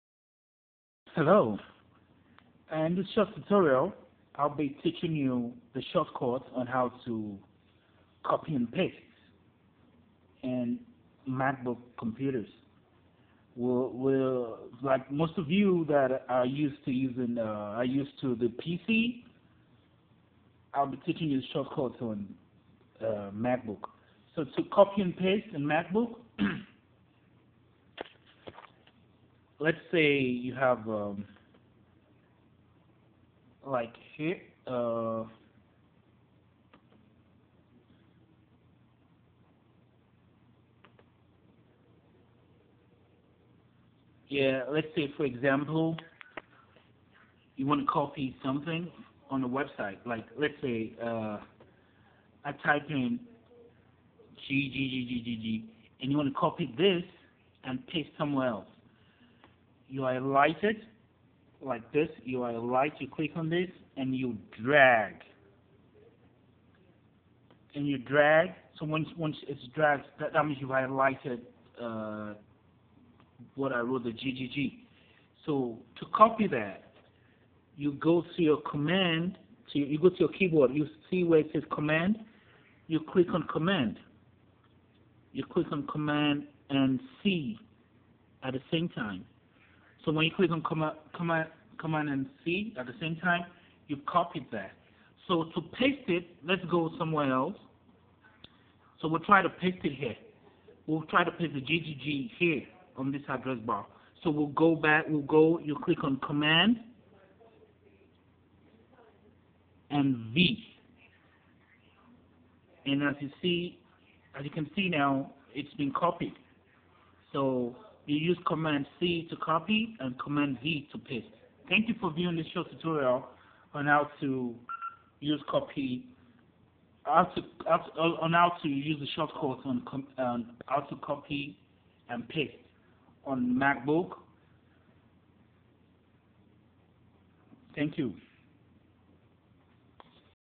How to copy and paste on MacBooks
Short tutorial  on how to copy and paste on MacBook